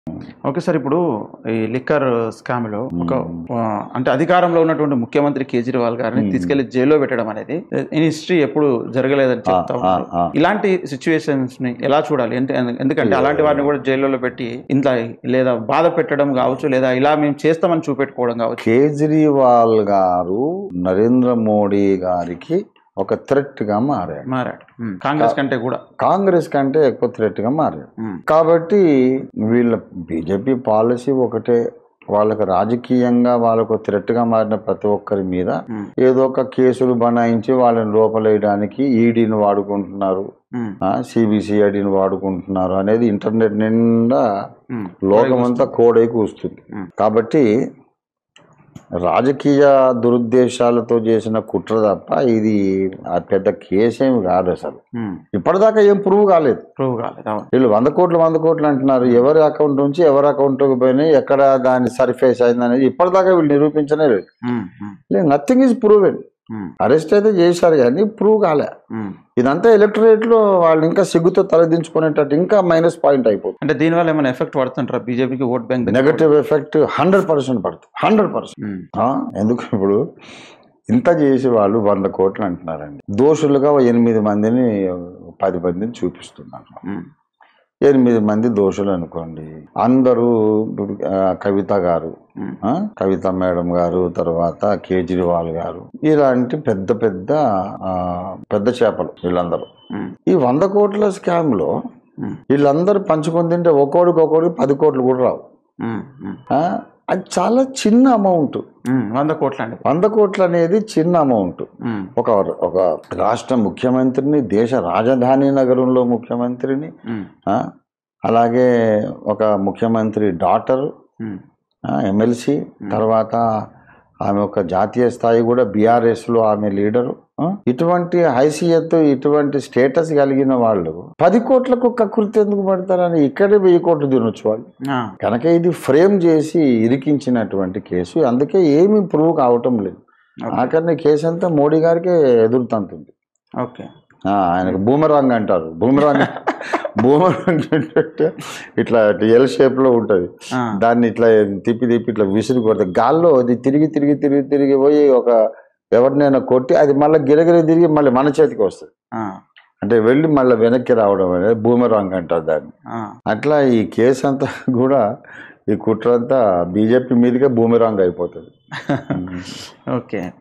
ksm: CM కేజ్రీవాల్ ని ముఖ్యమంత్రి హోదాలో అరెస్ట్ చేయడం పై ఓఫీర్ గారి స్పందన!
Yadartha Vaadhi Tv Programs:
►KSM "క్షీరసాగర మధనం" :

Eye witness: CH. Samuel Manohar Msgs
Ps. Issak Christian Folk Songs